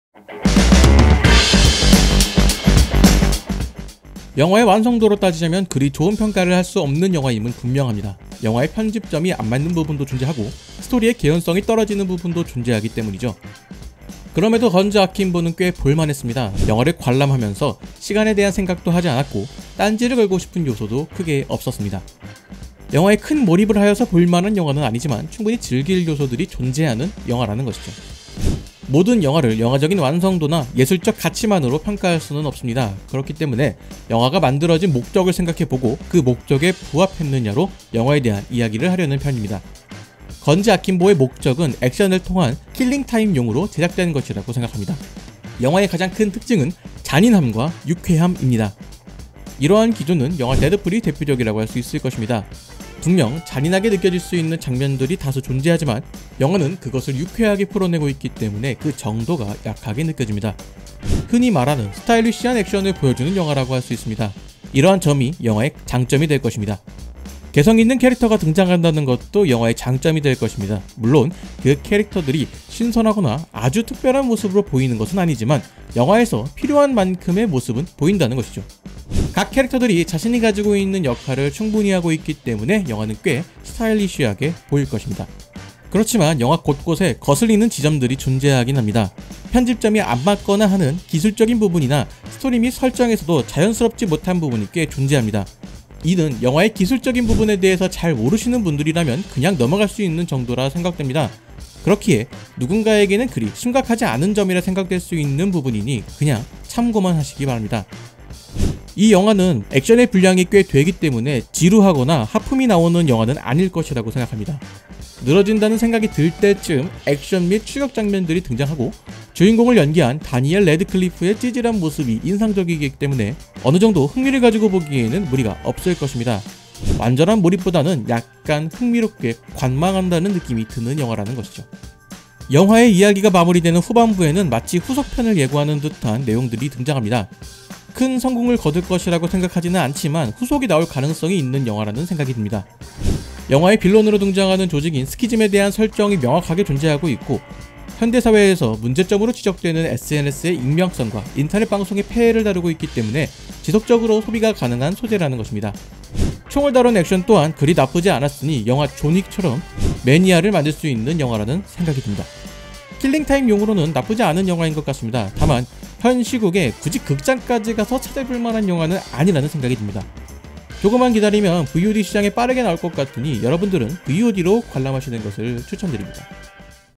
영화 리뷰 / [건즈 아킴보] / Guns Akimbo
단평 - 그냥 그런대로 볼만한 영화입니다. 굳이 영화관을 가야 싶긴 하지만, 다니엘 래드클리프의 유쾌한 모습이 매력적으로 다가오는 영화이기도 합니다. 망치를 휘두르는 장면에서 MC해머의 노래가 나오는 등 다양한 노래와 다양한 컬러와 현란한 카메라 워크로 사람들의 눈을 사로 잡는 영화입니다. 액션 연출 및 편집 등 기술적으로 부족한 부분이 보이긴 하지만 킬링타임으로는 볼만한 것 같네요.

건즈 아킴보 / 2020년 4월 15일 개봉

감독 : 제이슨 레이 하우덴
배우 : 다니엘 래드클리프, 사마라 위빙, 네드 데네히

이 영상의 자막은 Vrew를 이용해 제작하였습니다
이 영상에는 네이버의 나눔 글꼴 '나눔 바른펜', '나눔 고딕'이 사용되었습니다.
썸네일에는 배달의 민족 글꼴인 '배달의 민족 주아'가 사용되었습니다.